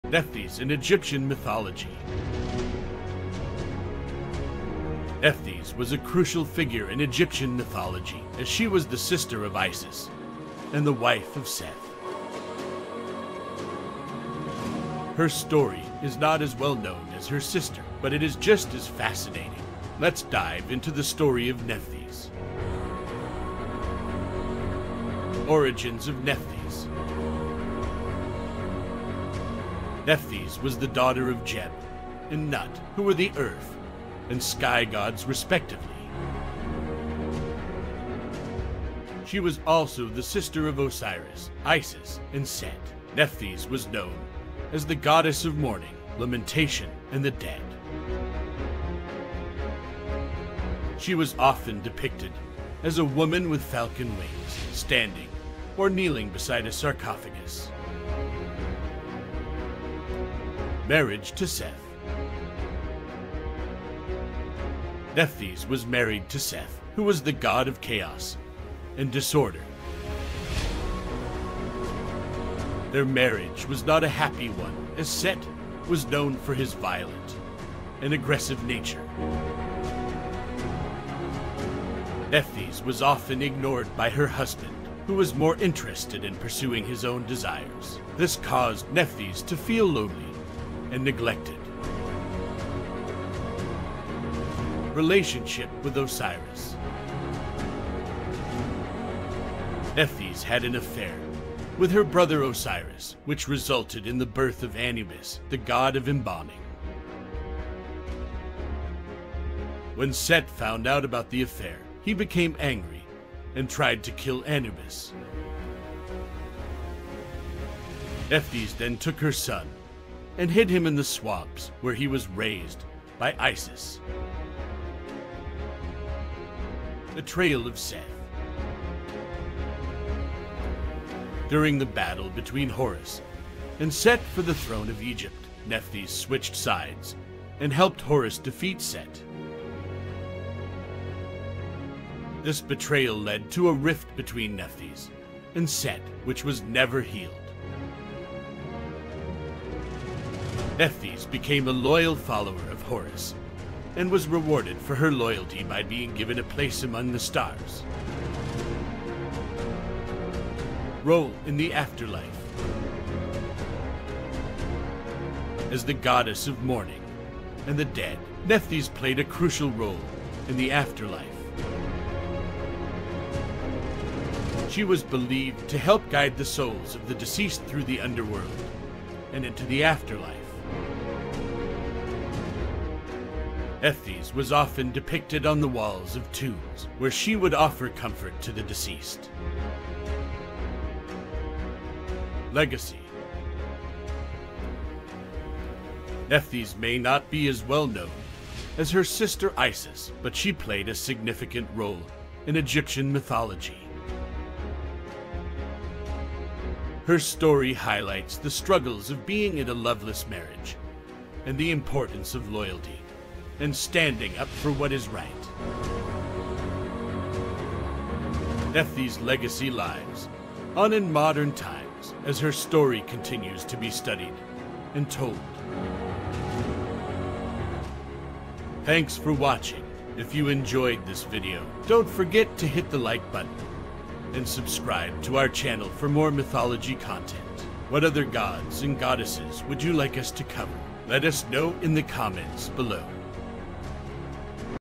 Exploring the Mythology of Nephthys: Goddess of Mourning and Funerary Rites
Join us as we explore the fascinating story of Nephthys, the goddess of mourning and the dead in Egyptian mythology. From her troubled marriage to Set, to her affair with Osiris and her betrayal of Set, Nephthys' story is one of love, loyalty, and standing up for what is right. Discover the legacy of this lesser-known goddess and her role in guiding the souls of the deceased to the afterlife.

 Copyright: All material of  this video belonged to Mythology AI